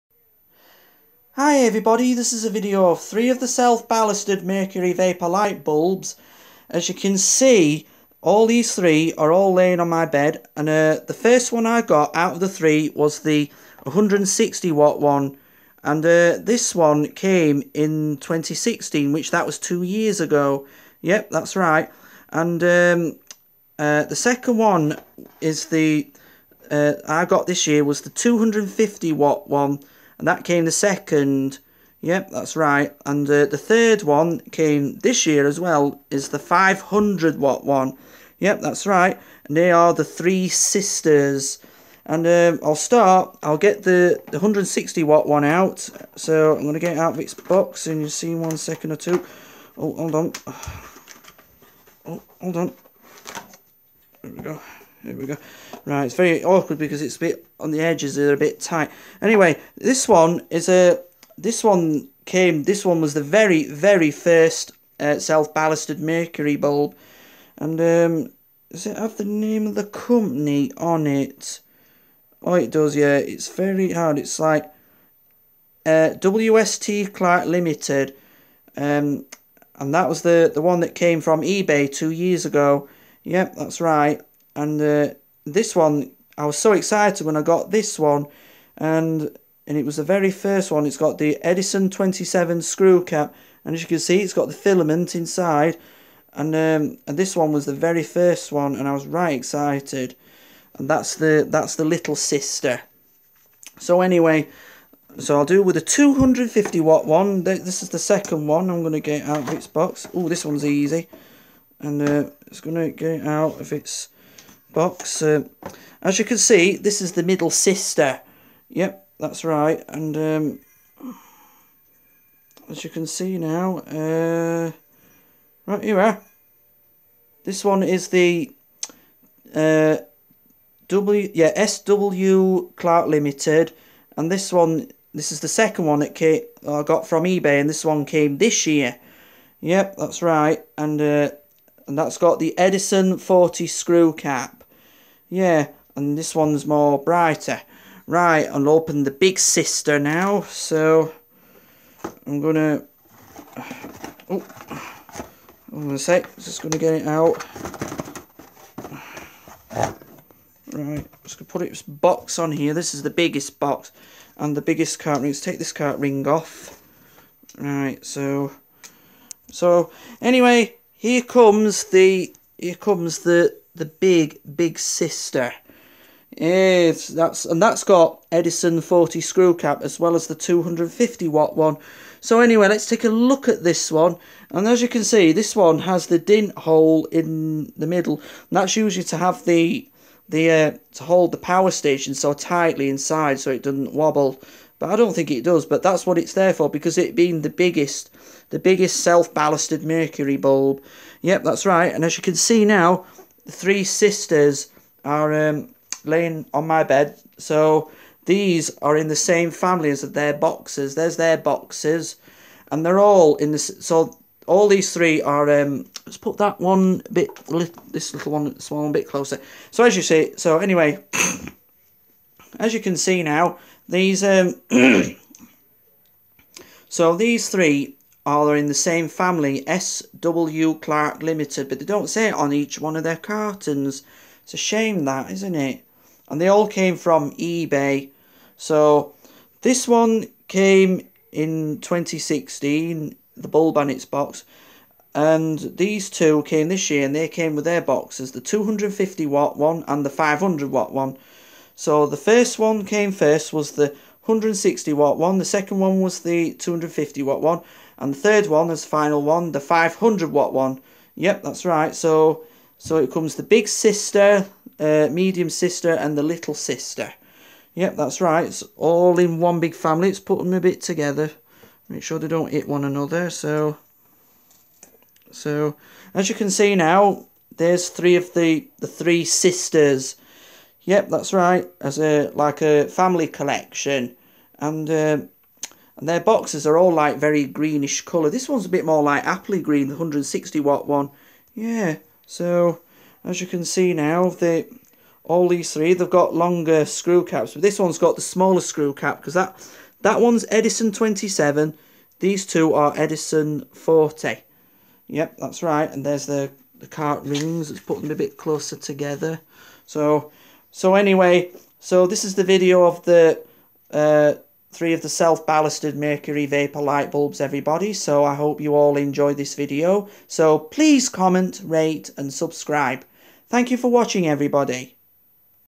3 self ballasted mercury vapor light bulbs 160, 250 and 500 watt
3 of the self ballasted mercury sisters which are in the same family.